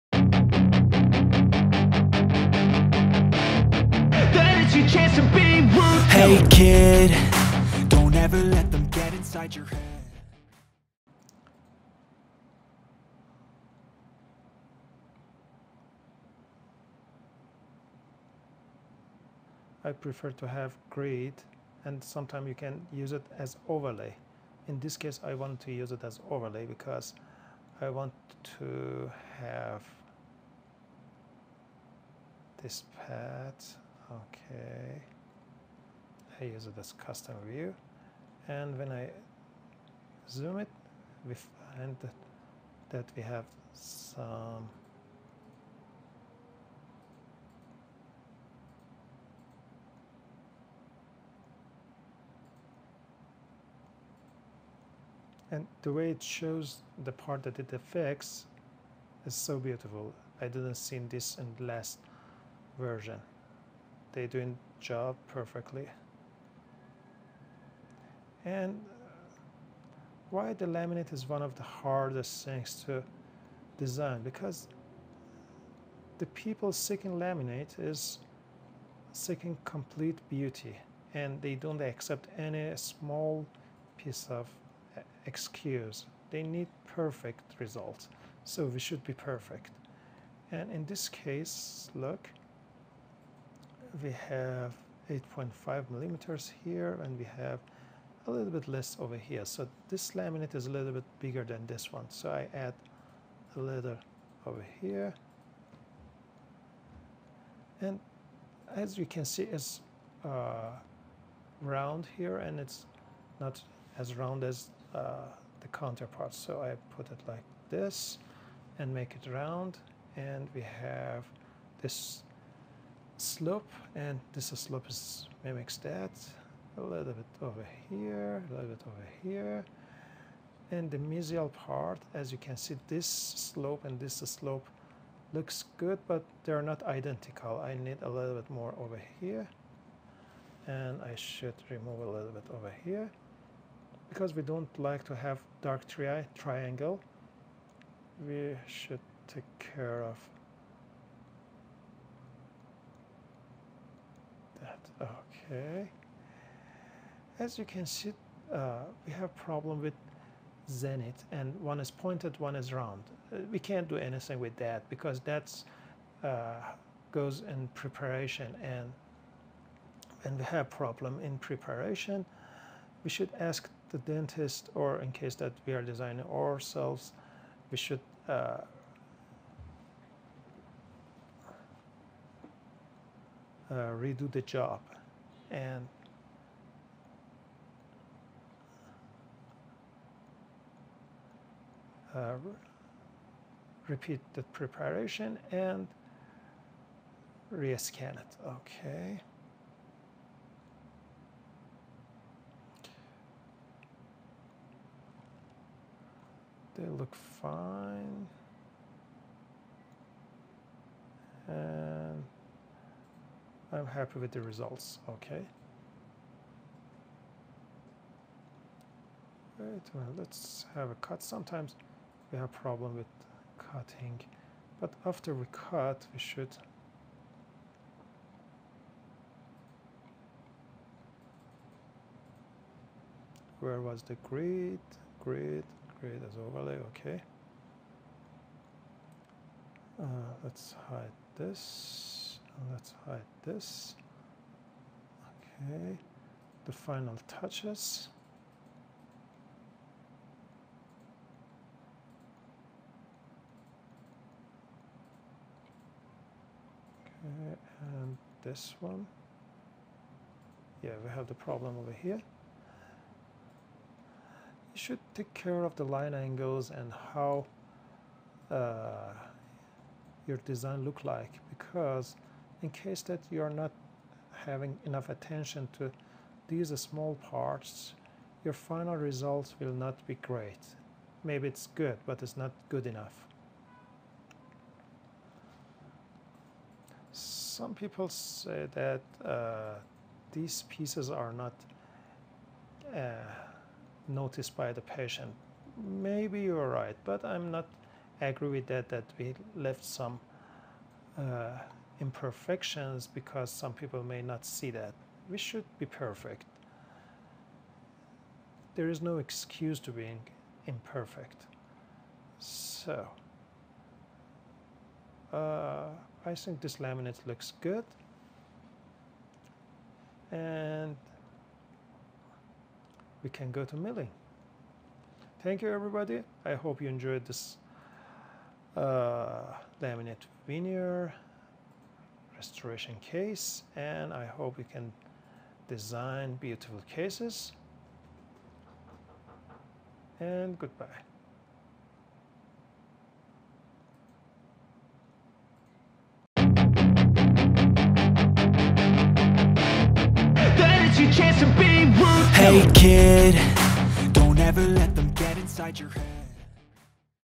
How to design laminate veneer with Exocad 3.1 Rijeka (part3)
In this video, we will teach you how to design laminate or reign with two different kinds of preparation first one is prepped  like a 3/4 crown, and one is just buccal side facing
▪️Making 3D printing the new standard for surgical excellence.
▪️Customized 3D models and surgical instruments.